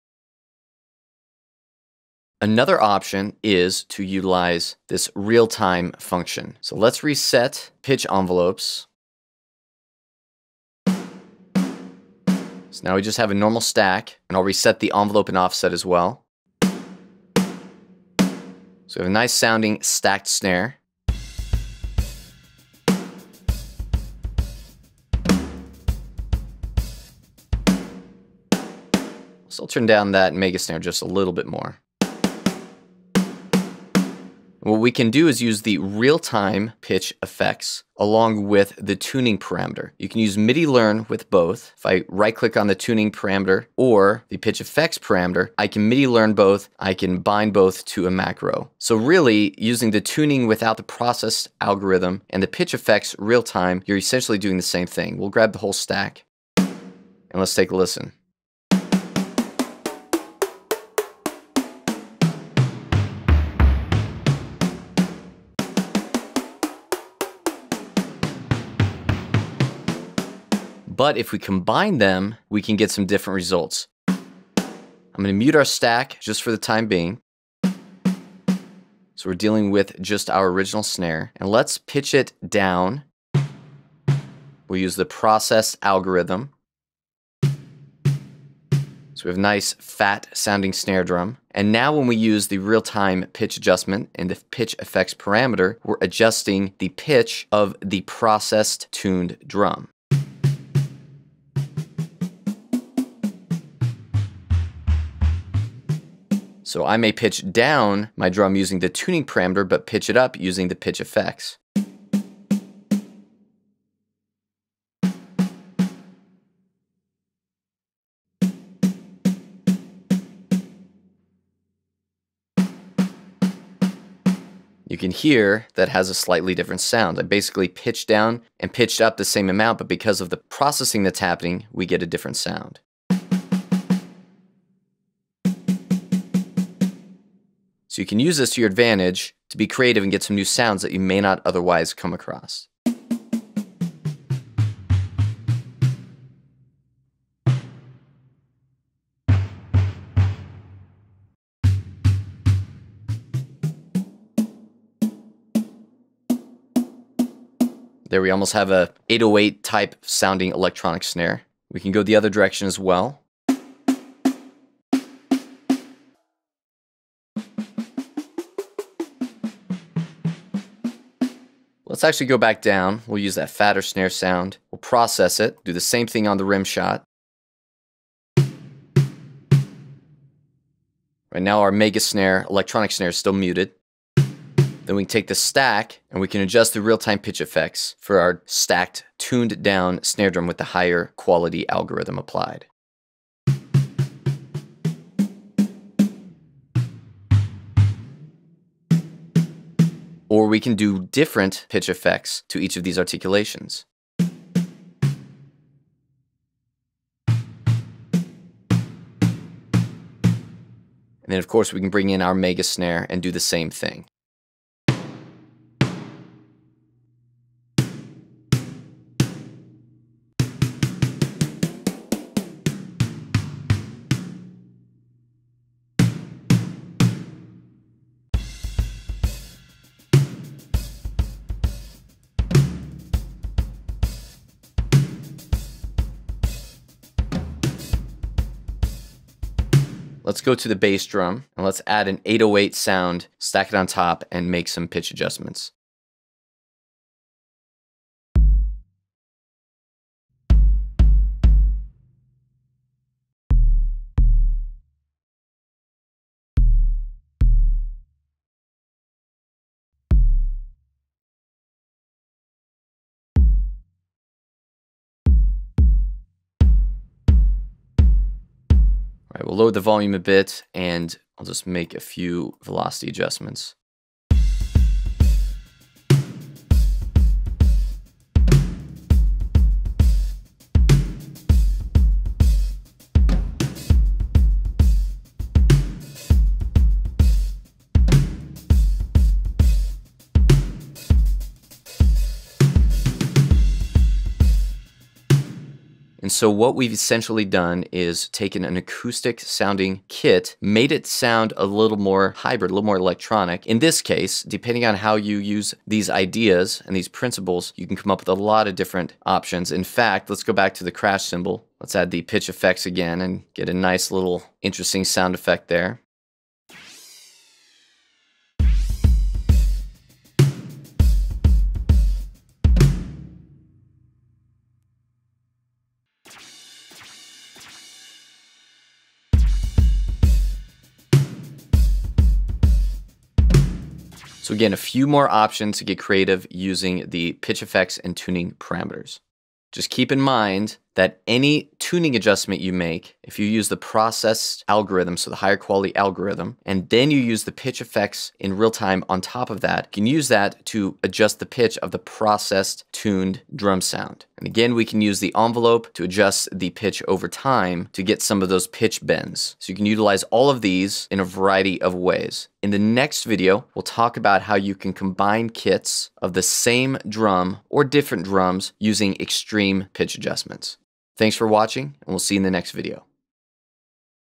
Creative Tuning Applications (Superior Drummer 3 Tips & Tricks Vol 1)
Luke explains and demonstrates how to use the Tuning parameter with the high quality algorithm to tune your drums in a realistic fashion while still applying the Pitch FX to the processed drum sound.

Tutorial taken from: Superior Drummer 3 Tips & Tricks Vol 1